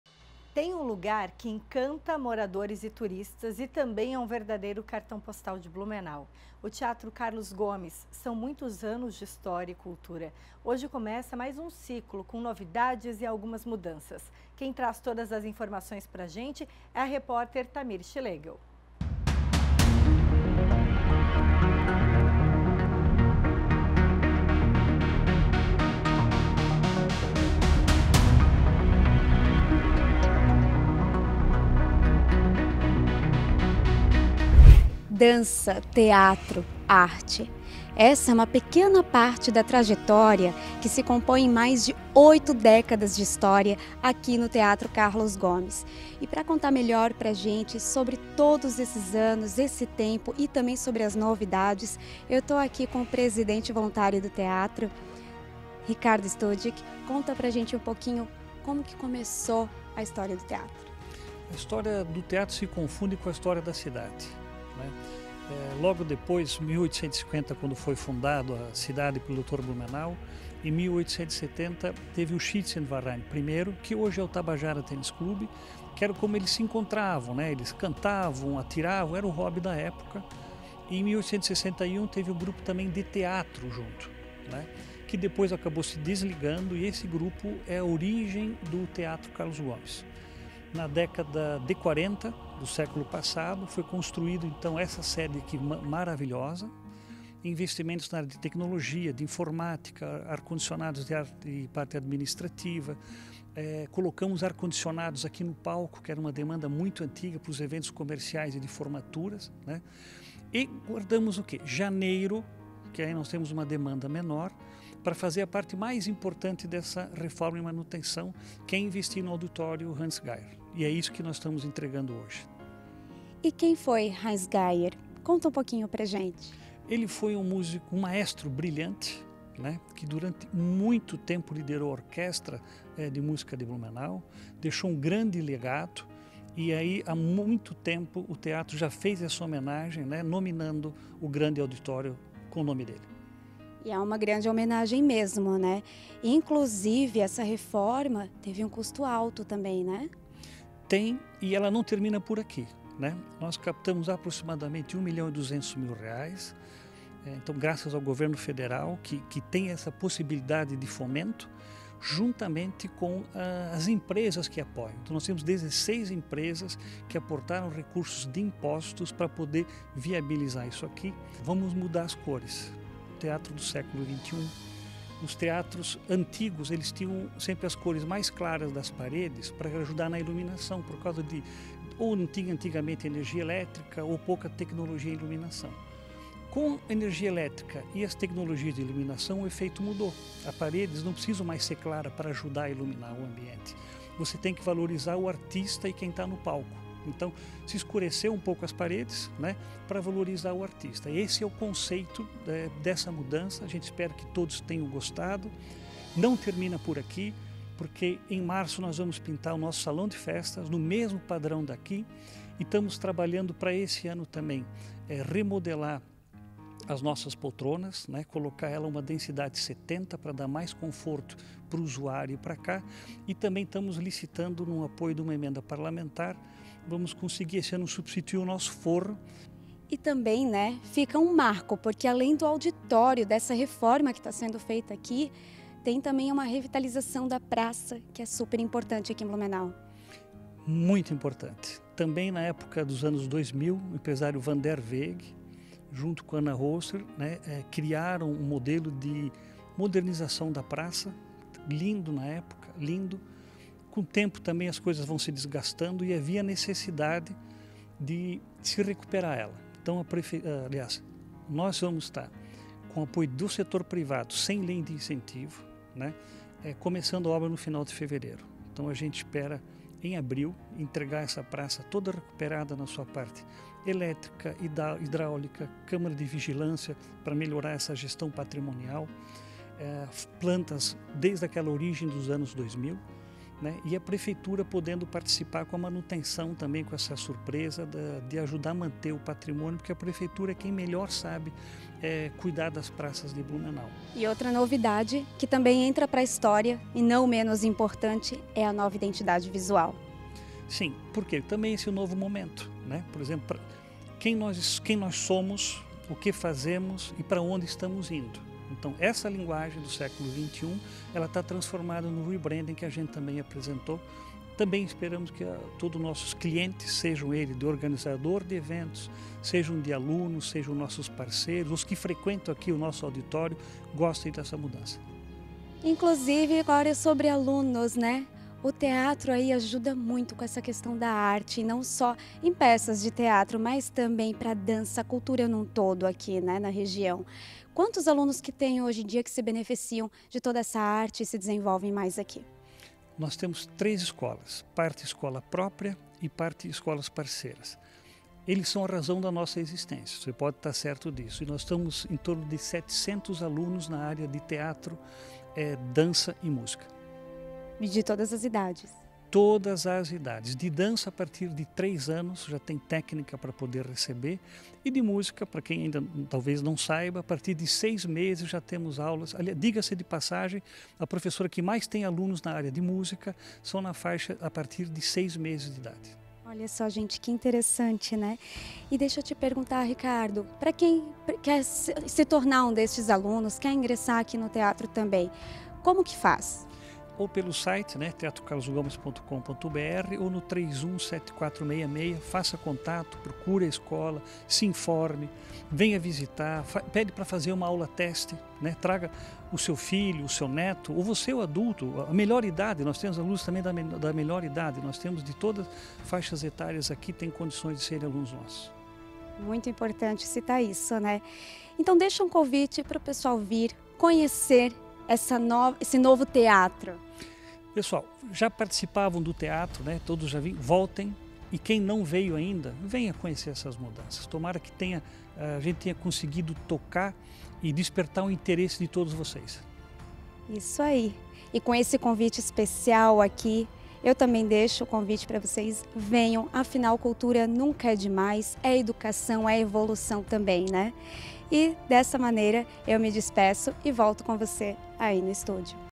Cartão postal de Blumenau: Praça do Teatro Carlos Gomes será revitalizada
Tem um lugar que encanta moradores e turistas e também é um verdadeiro cartão postal de Blumenau: o Teatro Carlos Gomes. São muitos anos de história e cultura. Hoje começa mais um ciclo, com novidades e algumas mudanças. Acompanhe!